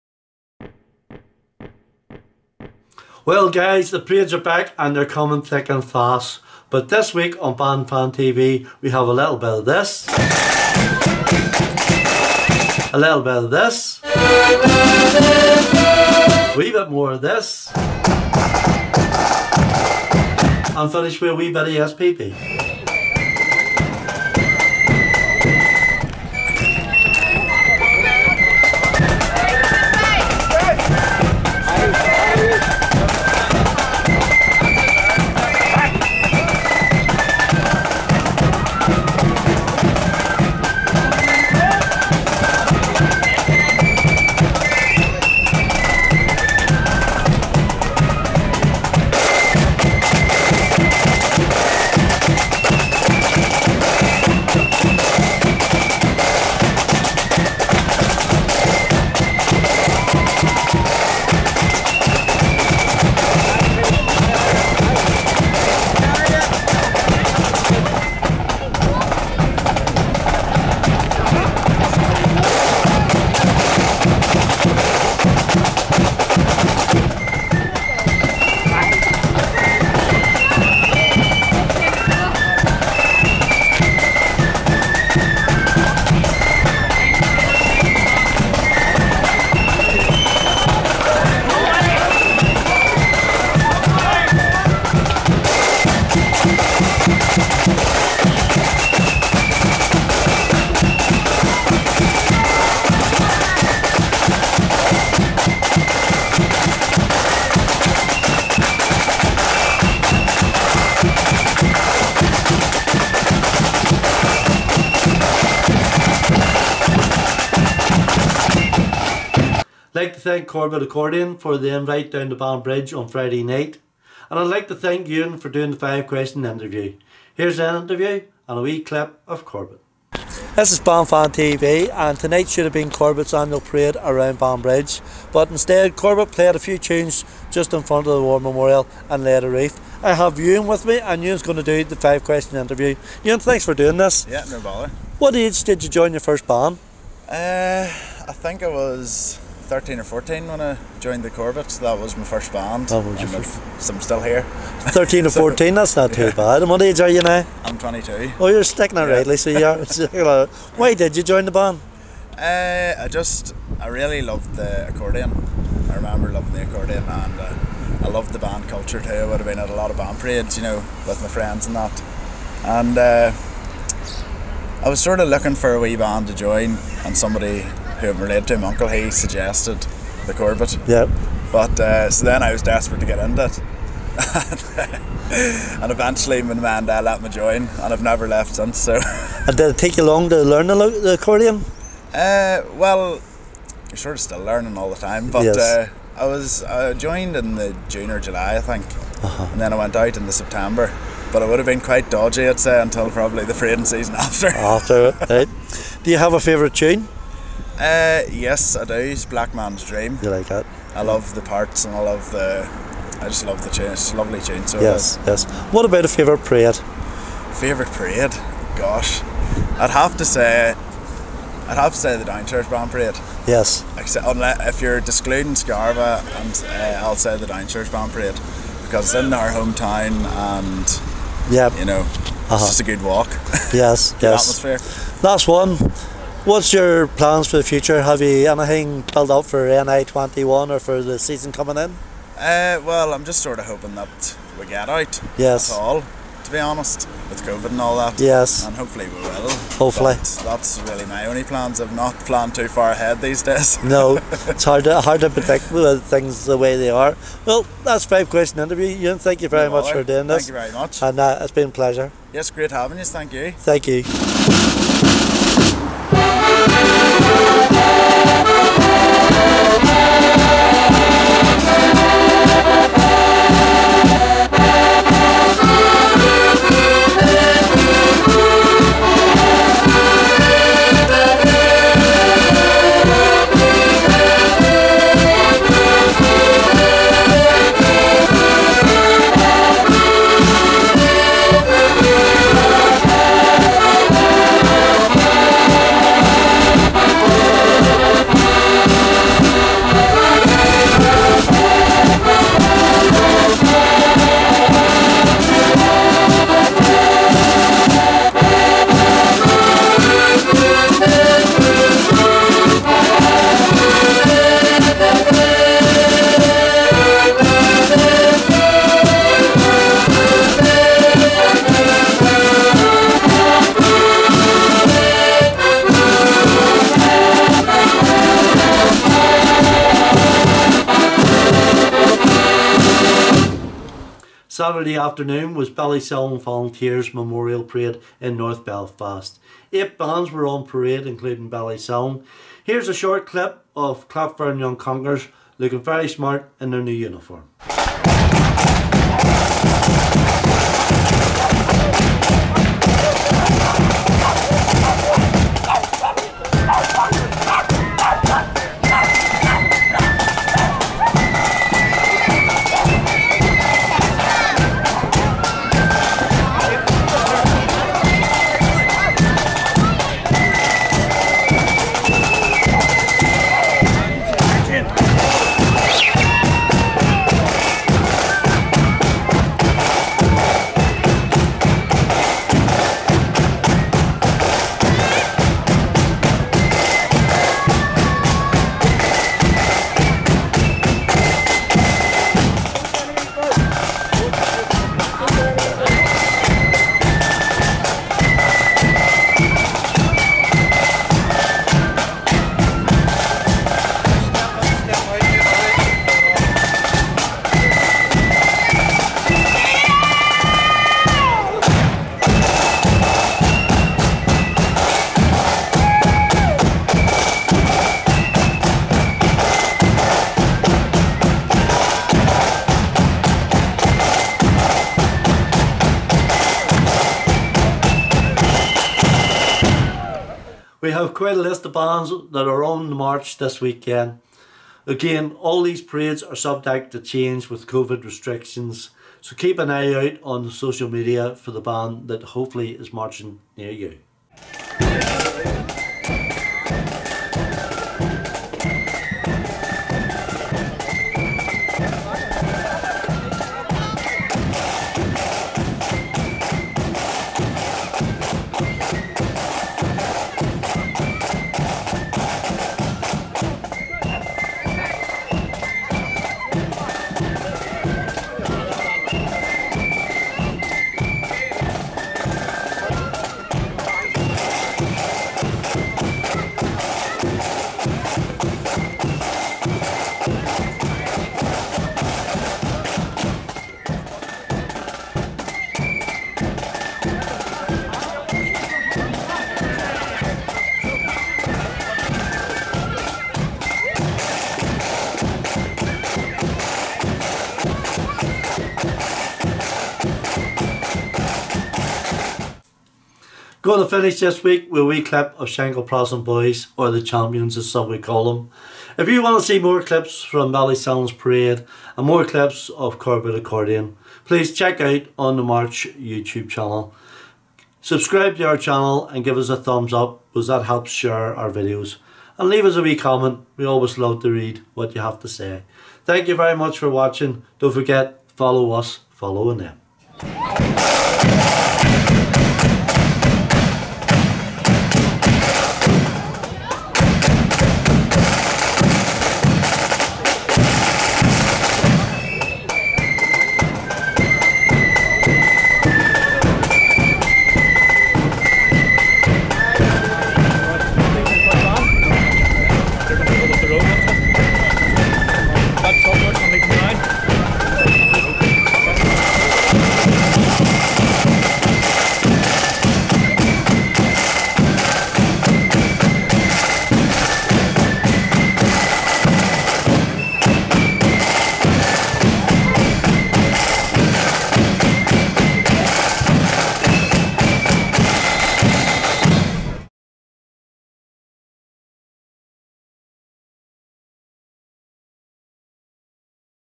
Band Fan TV no92 25/05/2021
This week on Band Fan TV we have clips from Ballysillan Volunteers Memorial parade and a interview and video of Corbet as they mark the night of their parade in Banbridge.